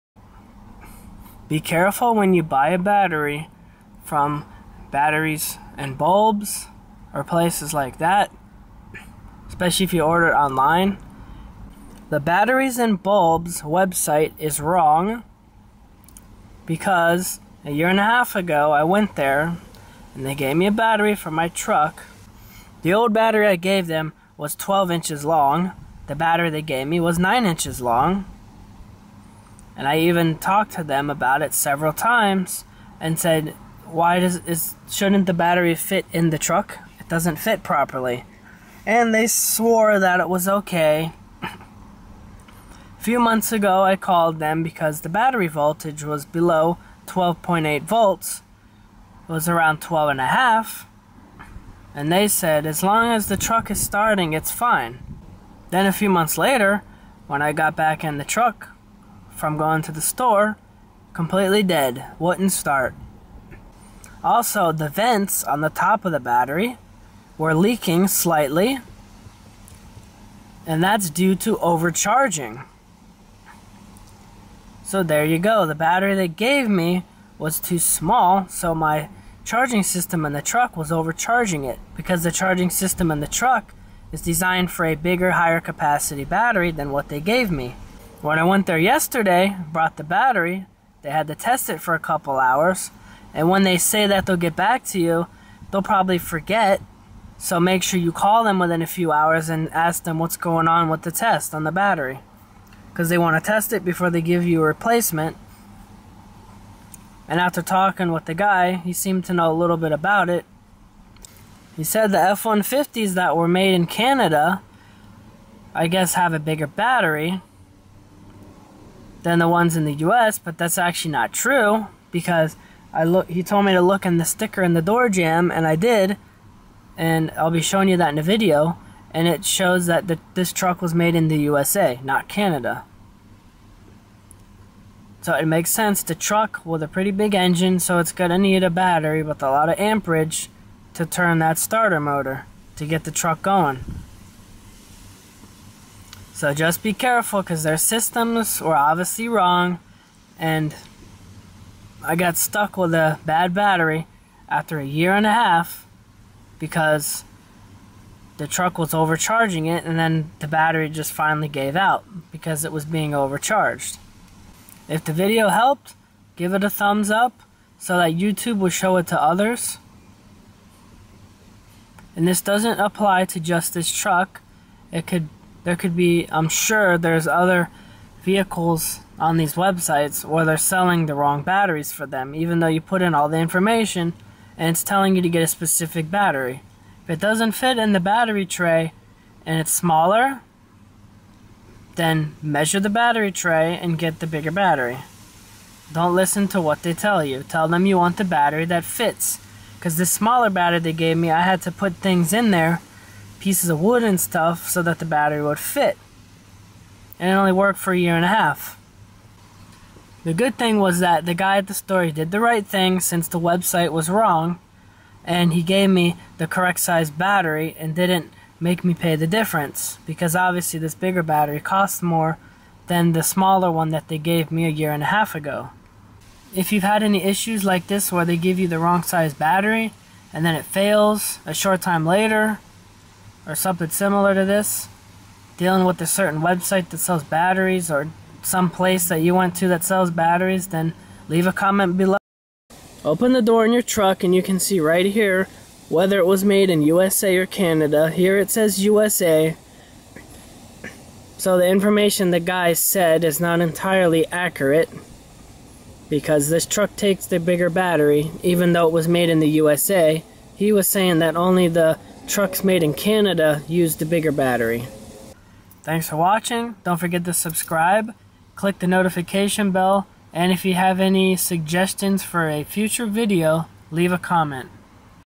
Beware ordering car batteries online
Websites like batteries and bulbs have the wrong size batteries listed on their websites be careful.